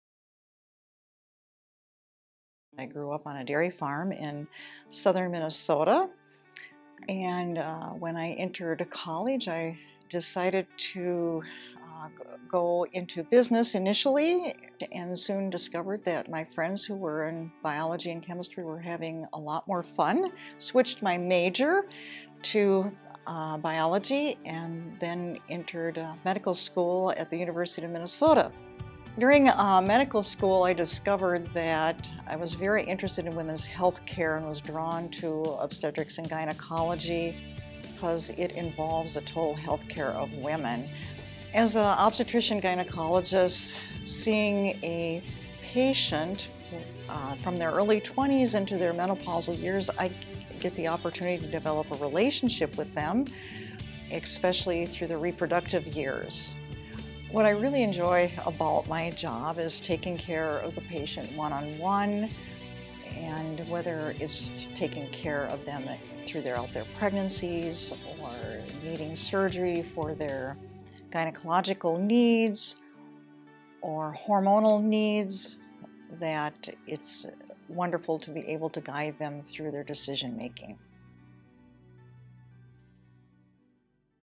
CentraCare Clinic - Vickie Nash
Meet Vickie Nash, MD, obstetrician/gynecologist.